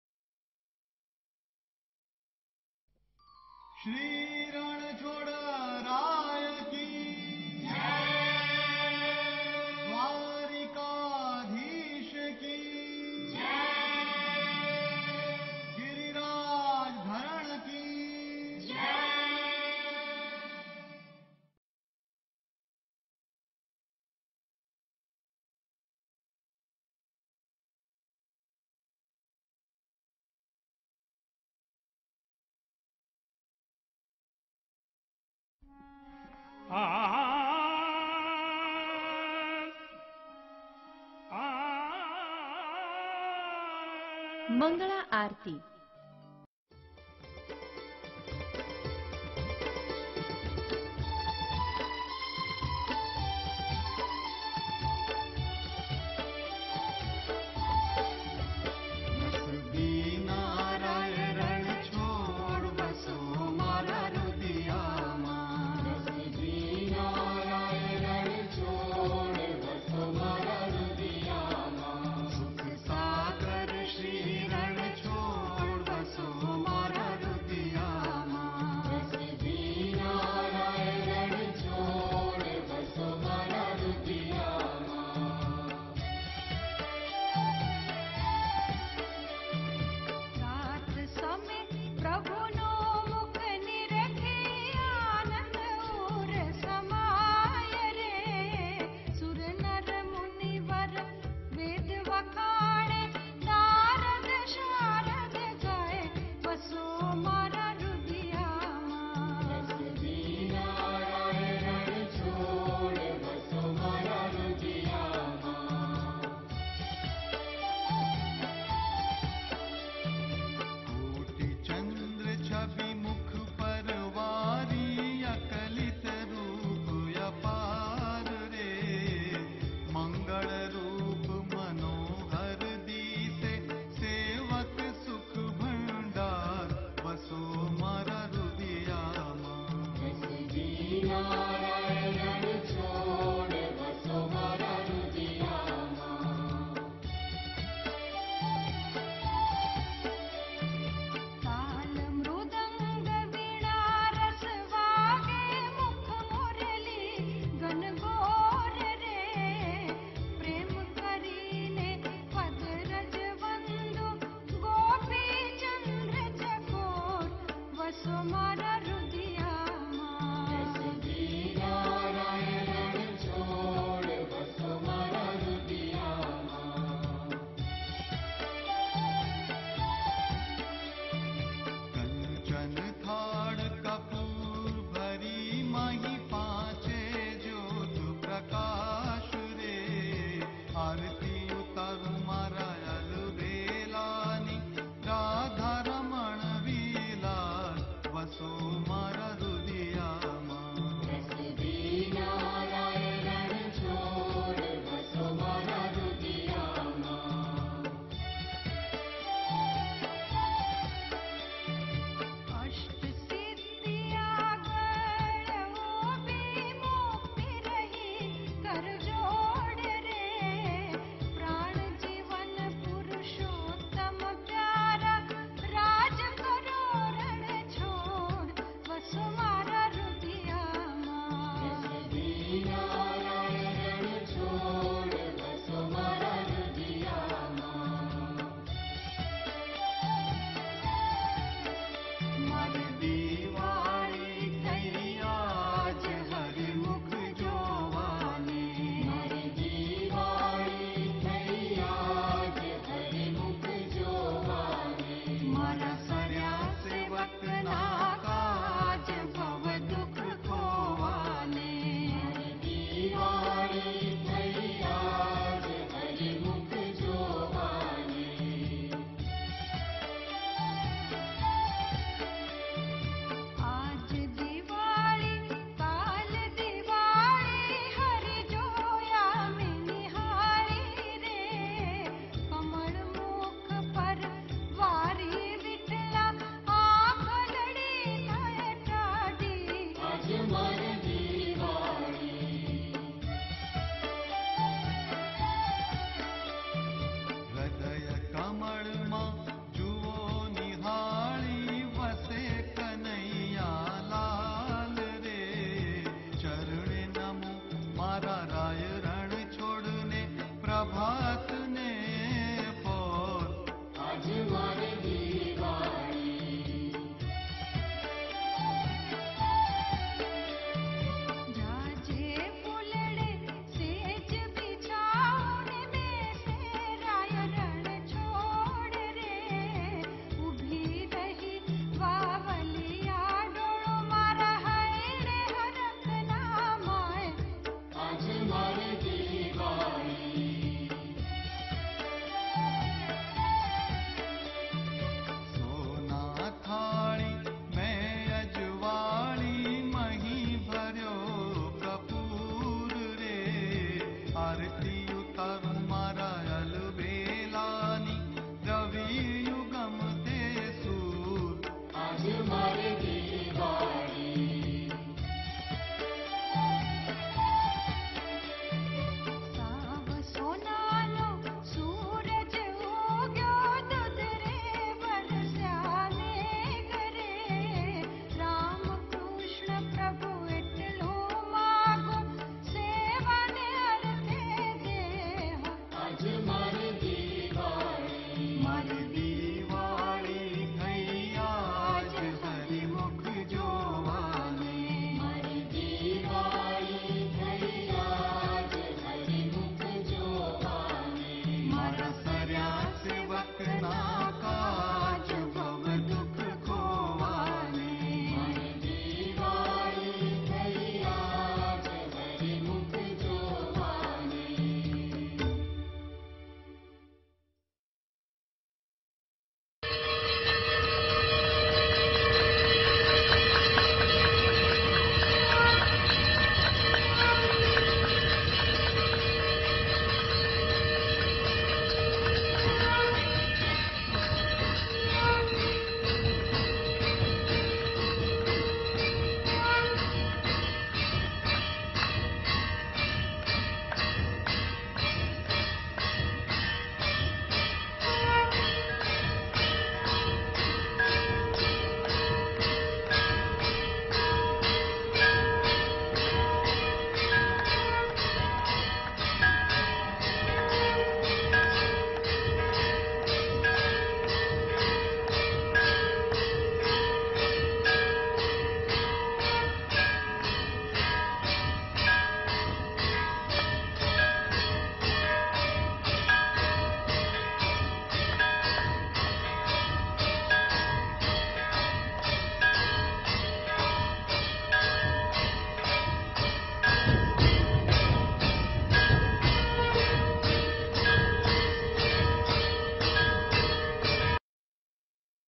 મંગળા આરતી દર્શન - જય રણછોડ રાય - DT - 04 - 10 - 2024 શુક્રવાર
ૐ कृष्णाय वासुदेवाय हरये परमात्मने।
प्रणतः क्लेशनाशाय गोविंदाय नमो नमः।।
આજરોજ ના શ્રી રણછોડરાયજી મંદિર 🛕 ડાકોર ના ઠાકોરજી ના 🪔 મંગળા આરતી દર્શન 🪔 🙏
DT - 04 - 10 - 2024 શુક્રવાર